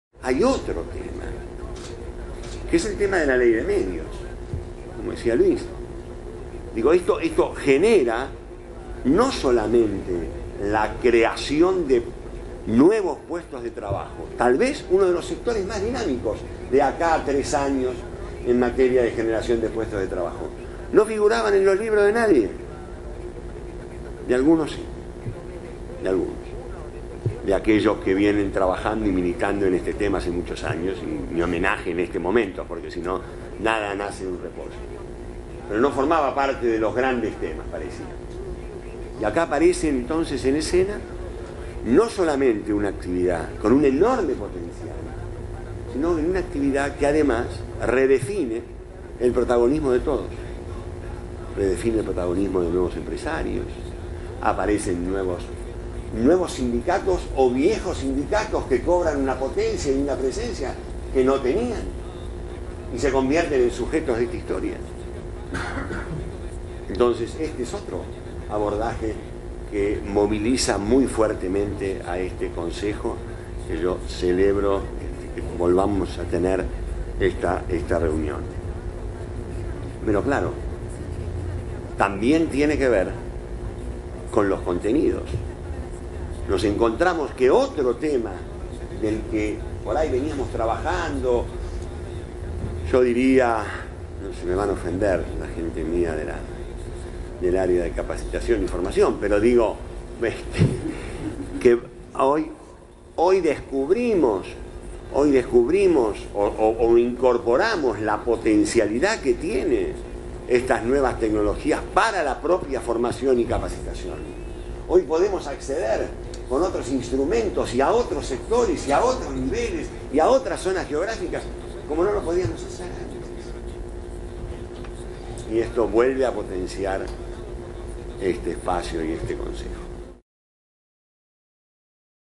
"La Ley de Medios redefine el protagonismo de todos".
El ministro de Trabajo, Carlos Tomada, destacó que la Ley de Servicios de Comunicación Audiovisual "redefine el protagonismo de todos", a partir del fortalecimiento de algunos actores y la aparición de otros  nuevos.
Así lo manifestó durante la reunión del Consejo Sectorial de Formación Continua y Certificación de Competencias de Industrias Culturales.
Del encuentro también participó el Coordinador General de la Autoridad Federal de Servicios de Comunicación Audiovisual, Luis Lázzaro, quien remarcó que "la ley establece una serie de principios que se articulan de manera directa con el empleo".